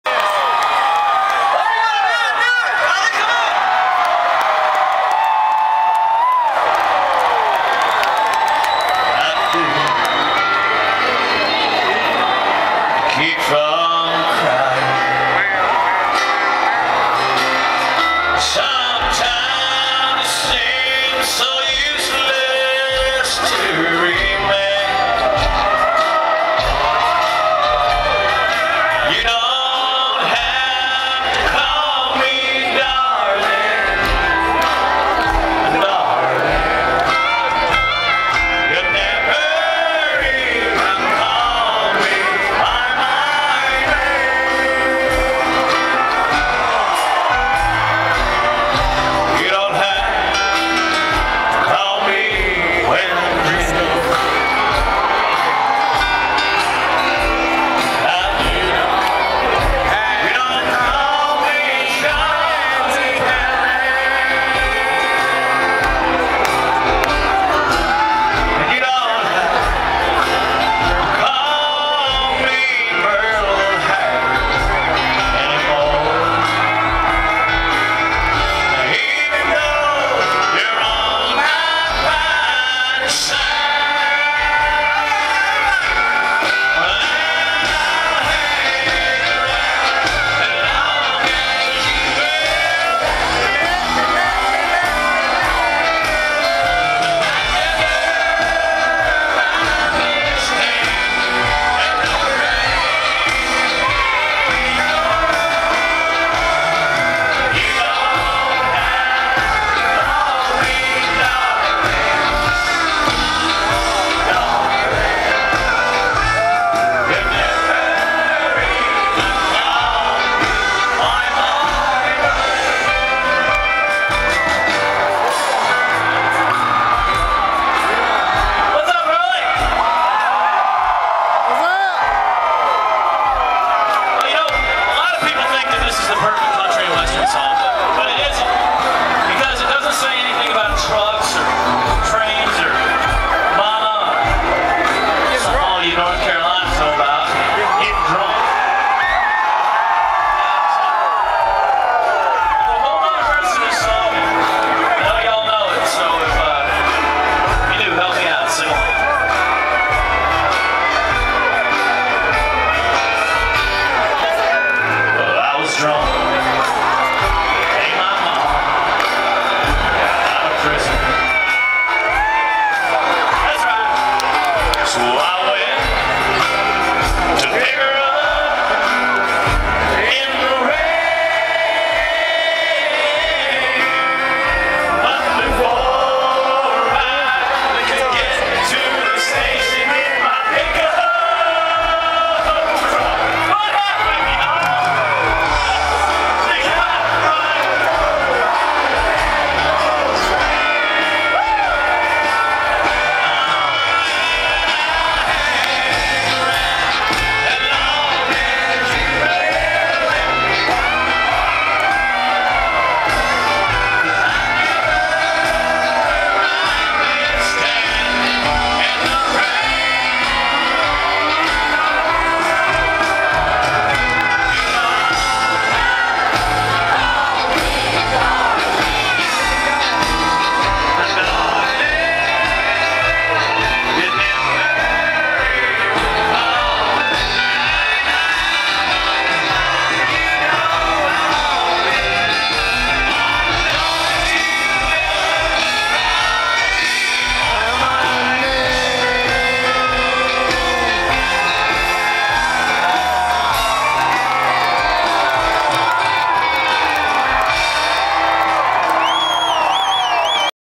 Kenny Chesney
Kenny Chesney Live in Raleigh singing David Allan Coe